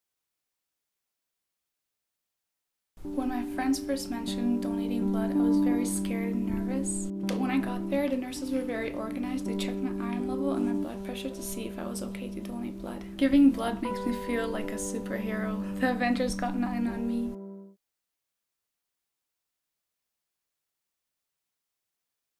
I was Scared
"I Donate" Campaign Designed Public Service Announcement Individual Project for 40-218 Production